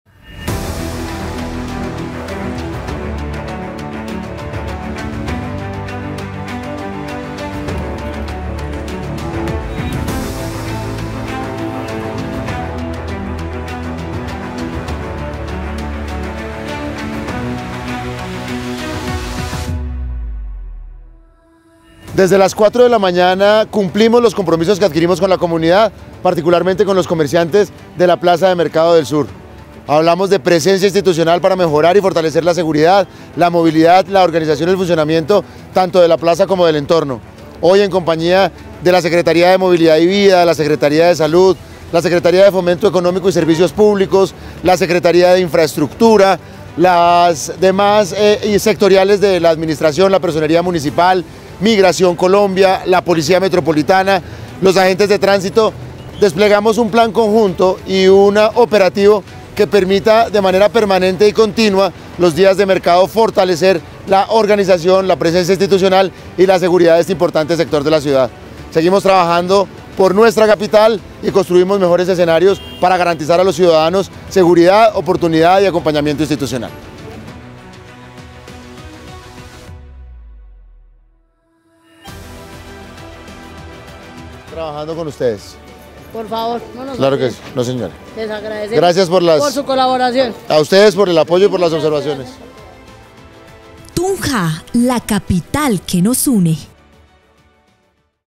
Acompañamiento institucional en la plaza de mercado del sur
Realizamos acompañamiento institucional en la plaza de mercado del sur, donde el alcalde Alejandro Fúneme, escuchó inquietudes y requerimientos de los comerciantes de este centro de abastos. Con los secretarios y funcionarios de la administración, seguiremos realizando control, verificación y garantizando seguridad para quienes trabajan y compran en la plaza.